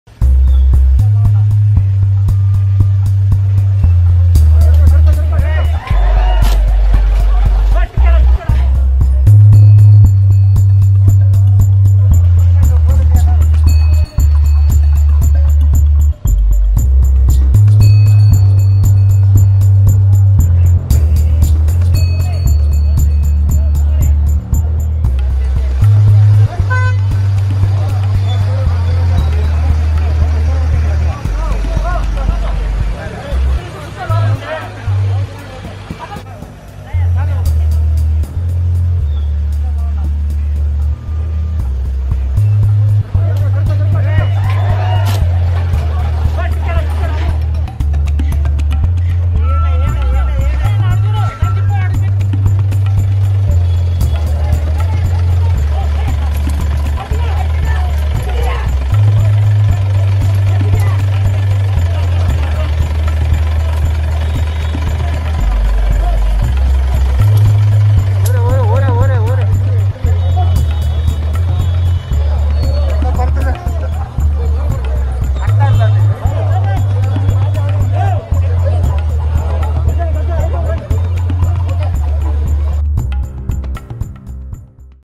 10-ft python coiled around tree brought down using a crane in Tirumala
A 10-foot long python was coiled around the branch of a tree. A snake catcher working for the TTD
rushed to remove the python with the help of a crane.